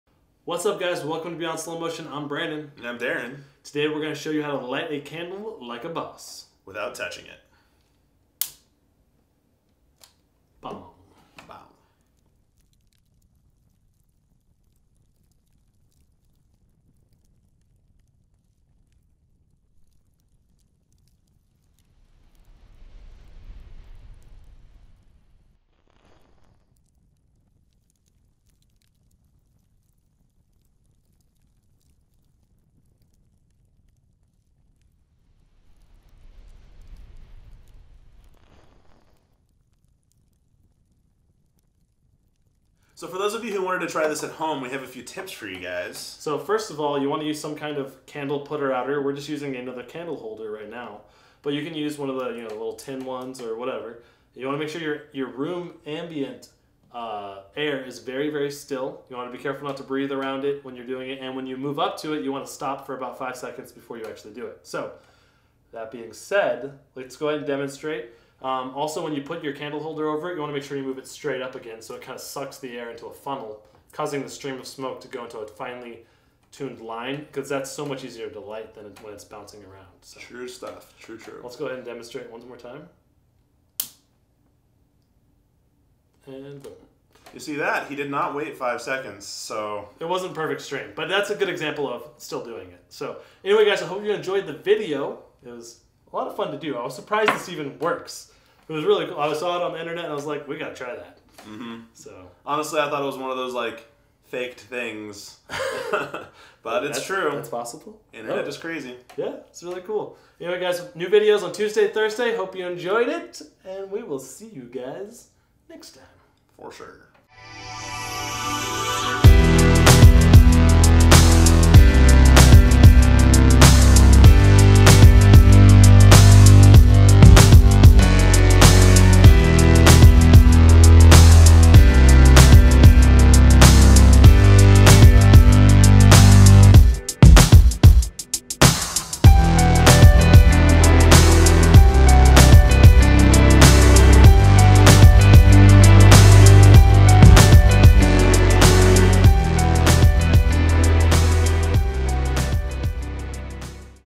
How To Light a Candle Like a Boss (480 FPS Slow Motion)
Well this was a fun experiment. Feel free to try this at home and leave some video responses! Make sure to tune in every Tuesday and Thursday for new slow motion episodes!